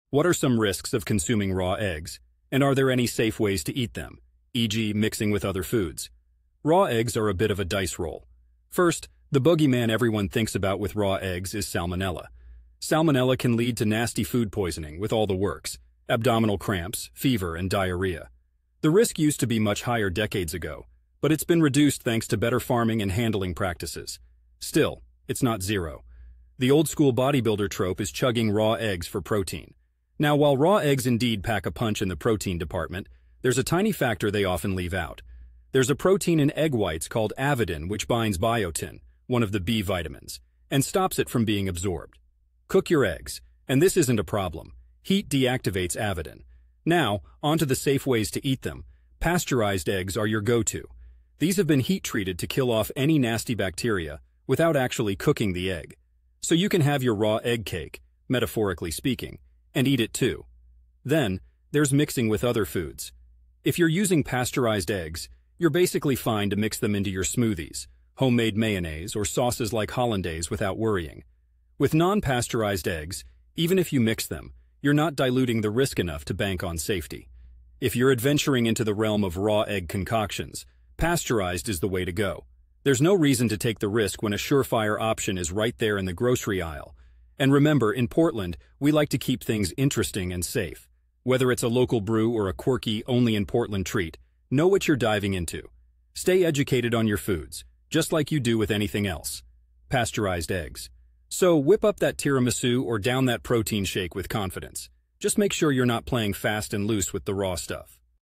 Risks of Eating Raw Eggs & Safe Consumption Methods?
I answered this question on Quora My video answer to the question: What Are Some Risks Of Consuming Raw Eggs Are There Any Safe Ways To Eat Them E G Mixing With Other Foods?